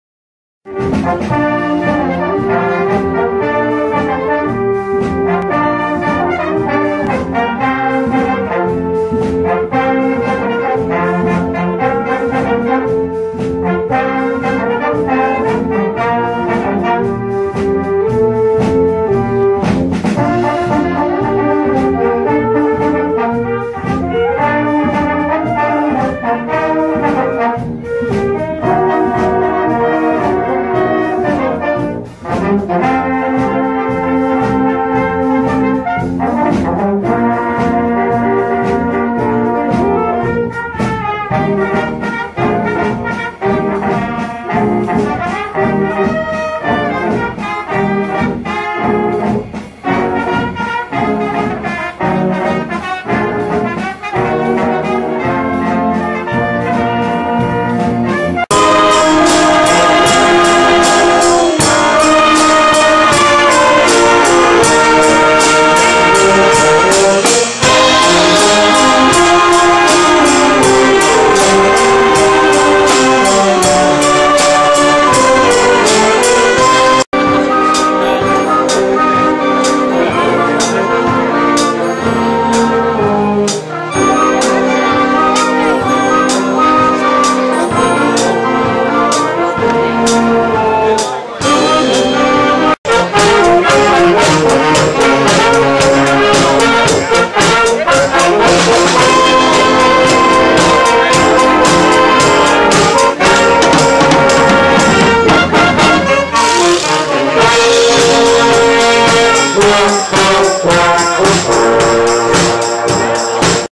Amigo Leiden Try out WMC Kerkrade 2017 fotoslideshow en optreden Stageband.
Fotoslideshow van de try out voor het WMC in Kerkrade en kort filmpje van de stage band van Amigo.